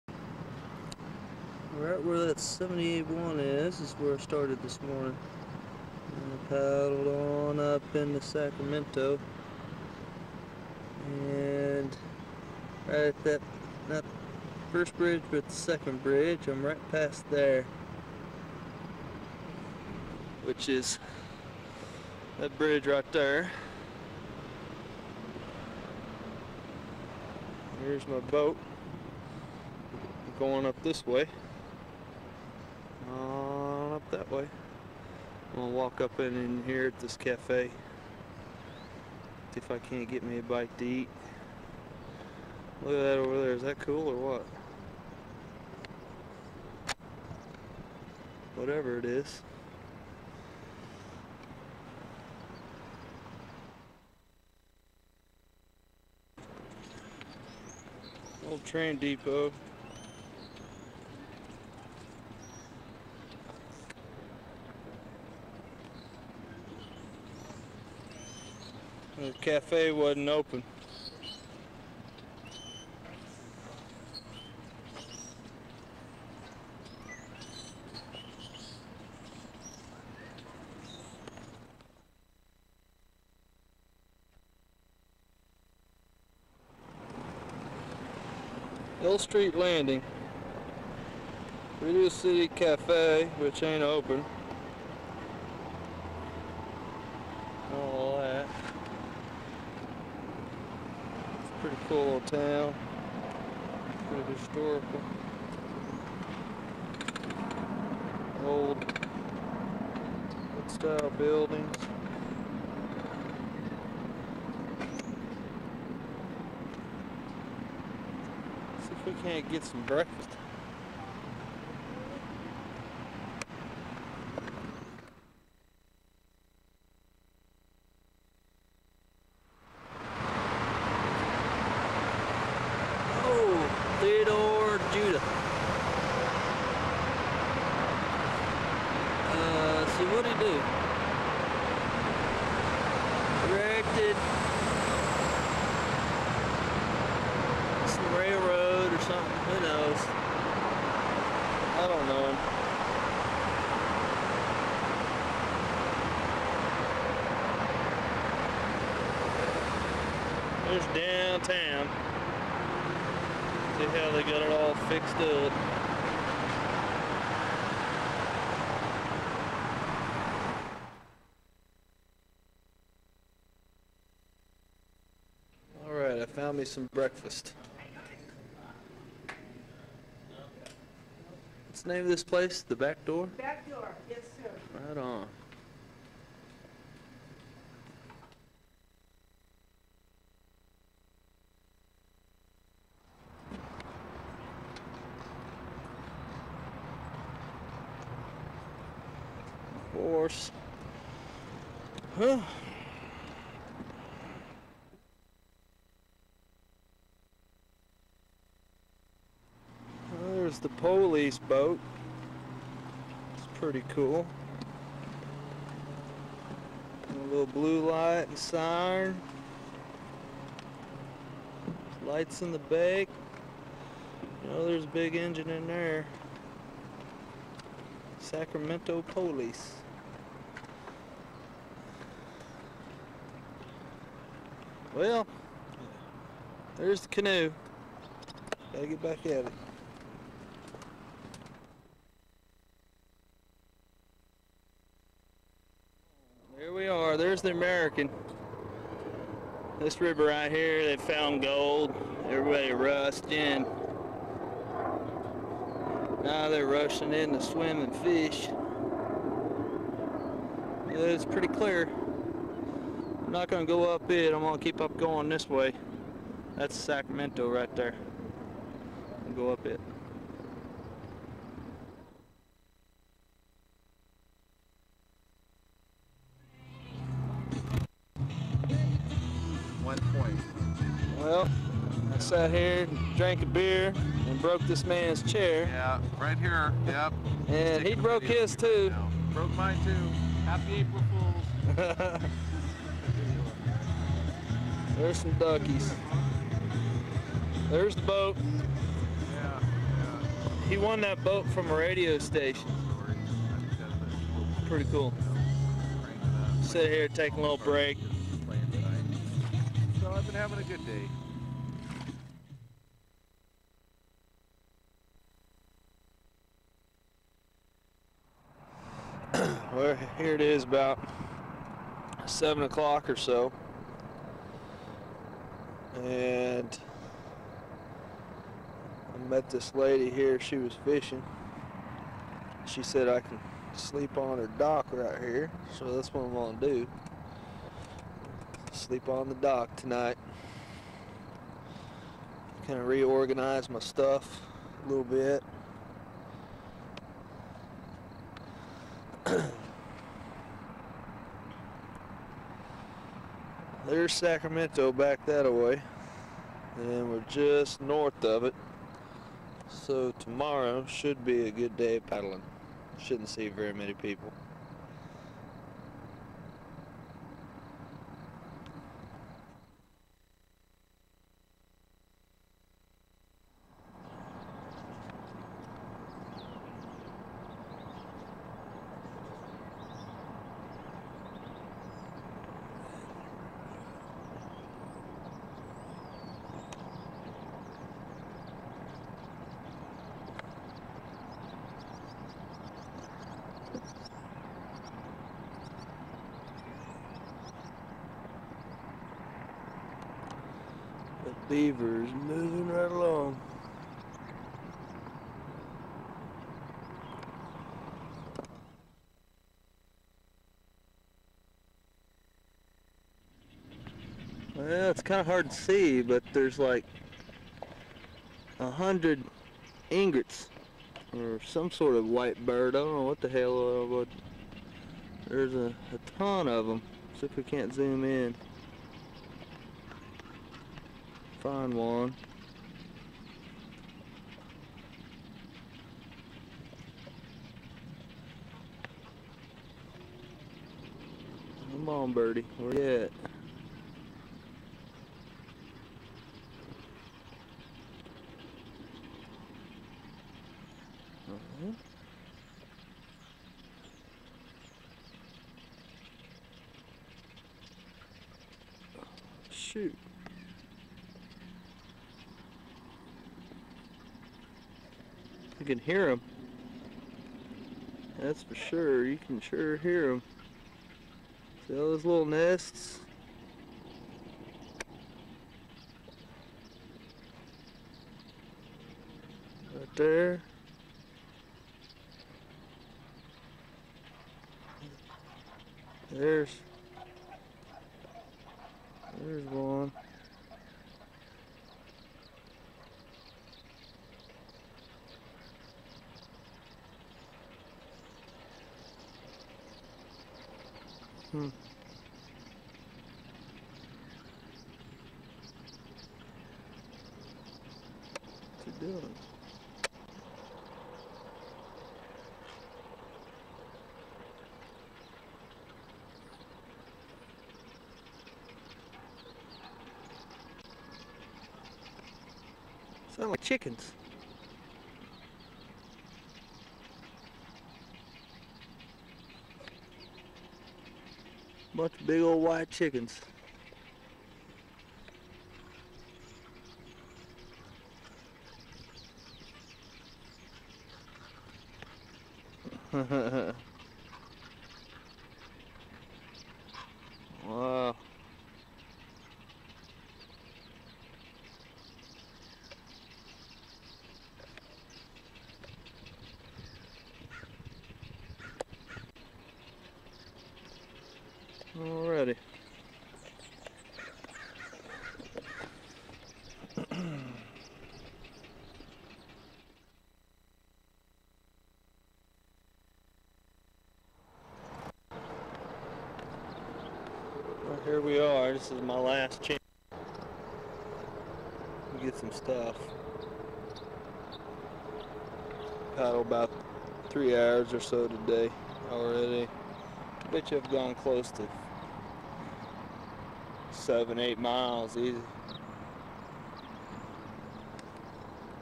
Rob paddles up the sacramento river- part 4
paddling up river, into northern california